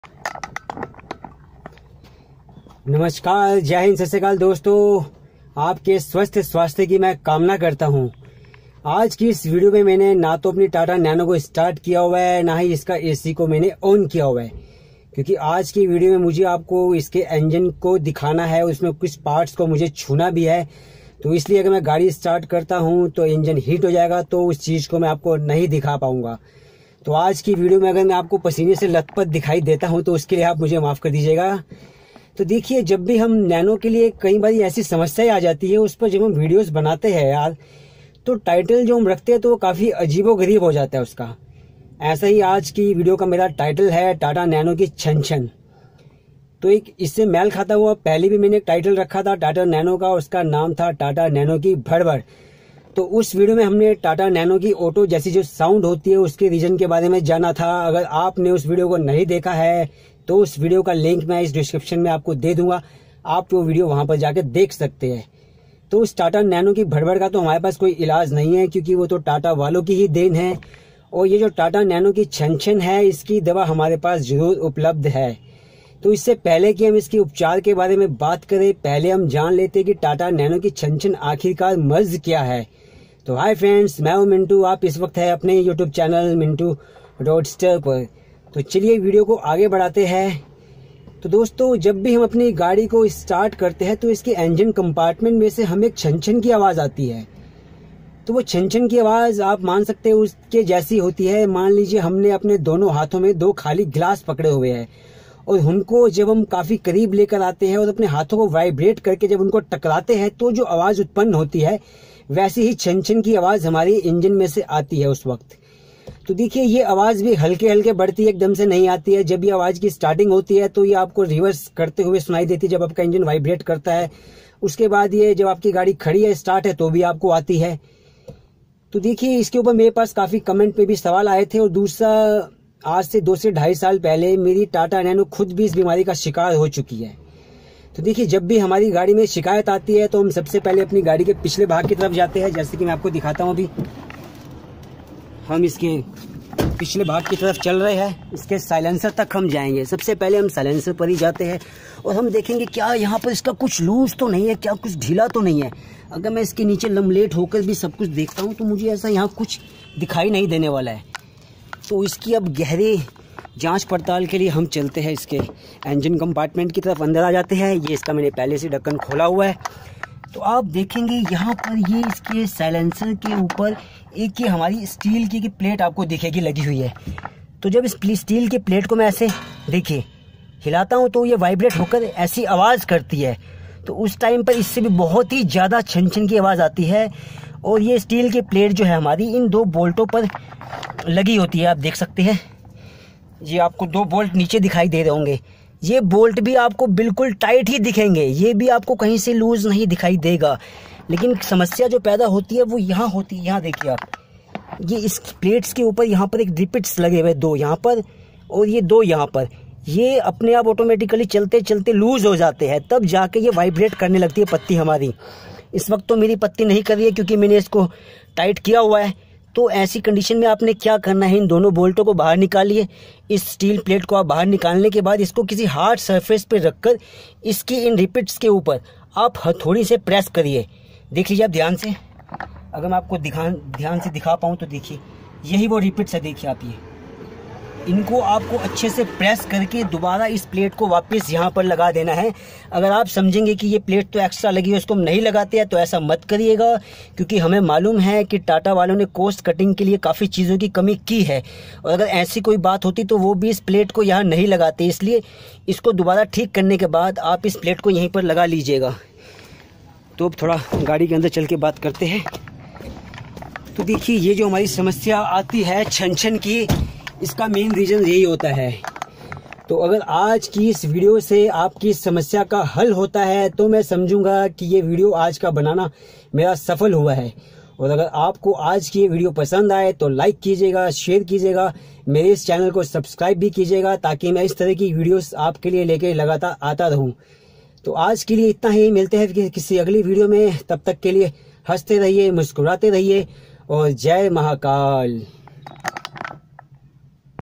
Tata nano ki Chan chan Tata motors mintoo roadster 😀
Tata nano ki Chan Chan aaj ki video mein hum BAAT karenge Tata nano mein aane wali ajeeb sound Chan Chan ke bare mein is video mein hum janenge ki ye problem kya hai aur iska solution kaise kare aur apni nano ko scielant Kar sake